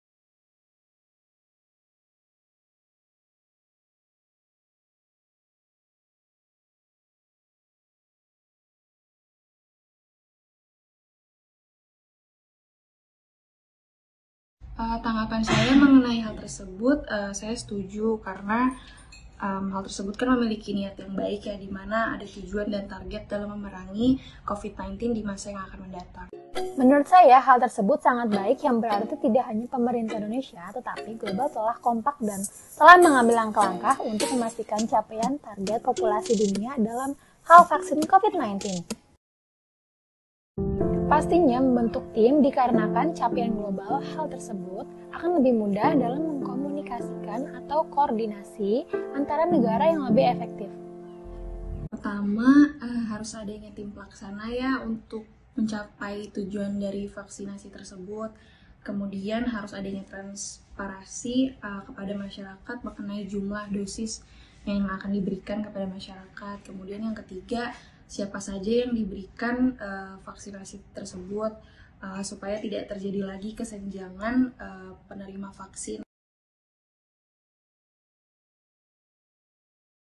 Vaksinasi Global Capai 70% pada Paruh 2022
Menteri Keuangan dan Kesehatan dari 20 ekonomi terbesar dunia (G-20) mengatakan akan melakukan langkah-langkah untuk memastikan 70% populasi dunia divaksinasi covid-19 pada paruh 2022 dan membentuk gugus tugas untuk memerangi pandemi di masa depan.

Apa kata masyarakat?